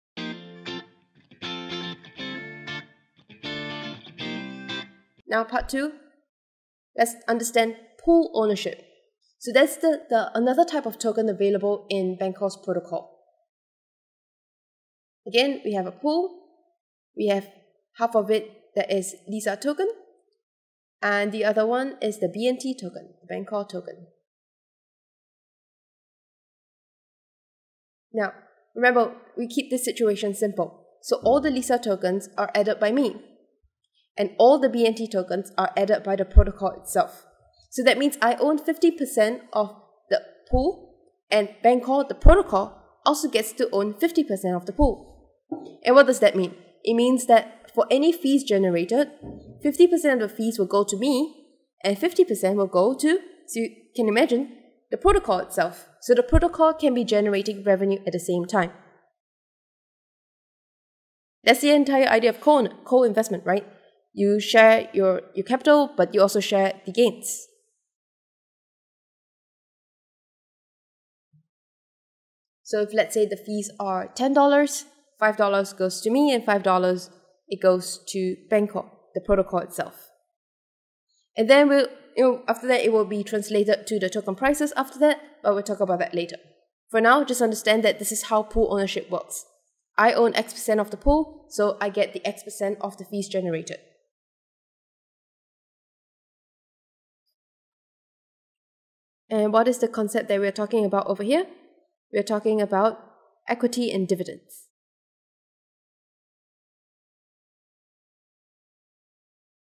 Pool Ownership in Bancor V2.1 || Explained using Equity and Dividends
In this video, we take a look at Pool Ownership.

Let's say we have a liquidity pool of which one side is filled by $LISA (added by me) and the other side is filled by $BNT ( added by Bancor).

That means I own 50% of the pool and Bancor owns 50%.

50% of ALL fees generated would go to me and the other half would go to the protocol itself, which is explained with an example in this video.

If you are from traditional finance, this is similar to the concept of Equity and Dividends.